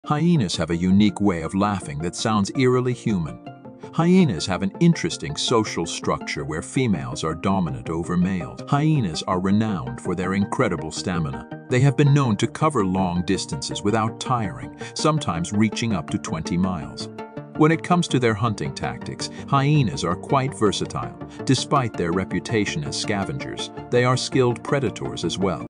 Hyena Chronicles: Secrets of the Wild Laughter Unraveled!
Get ready to embark on a thrilling adventure into the captivating world of hyenas! In this must-watch video, we uncover the hidden secrets and uproarious antics of these fascinating creatures. Discover the truth behind their bone-crushing jaws, their unique social hierarchy, and their legendary laughter that echoes through the African savannah. Join us as we delve into their wild hunting tactics, from cooperative strategies to cunning thefts from other predators. Brace yourself for jaw-dropping encounters and hilarious moments that will leave you in stitches. Are you ready to witness the untamed hilarity and uncover the enigmatic allure of hyenas?